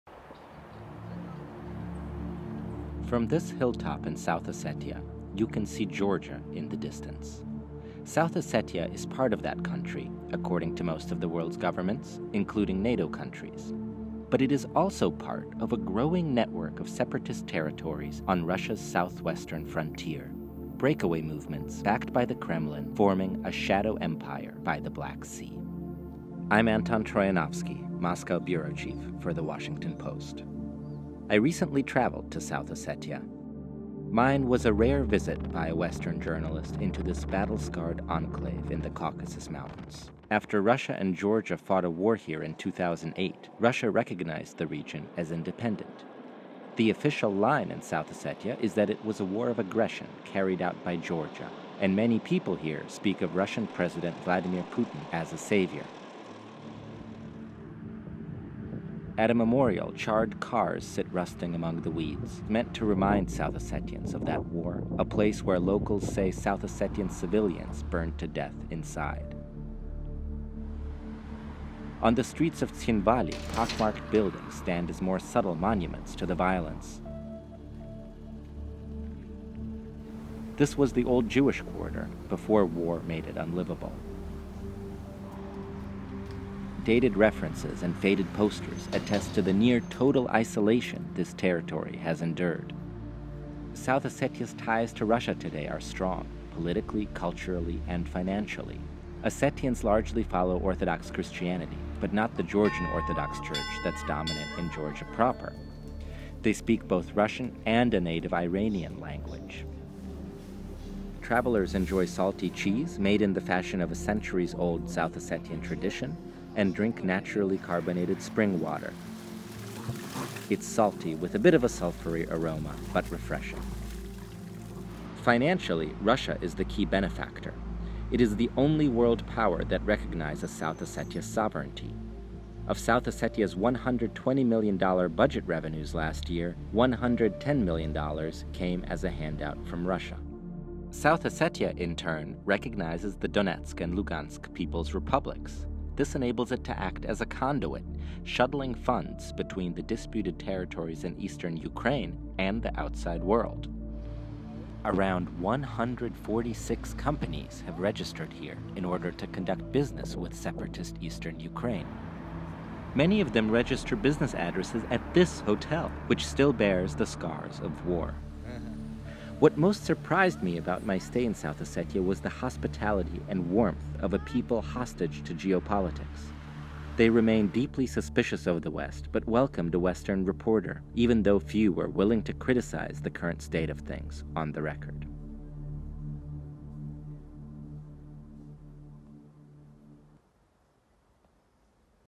This Russian-backed separatist enclave still bears the scars of war
The Post's Anton Troianovski traveled to South Ossetia, part of a growing network of separatist territories on Russia’s southwestern frontier.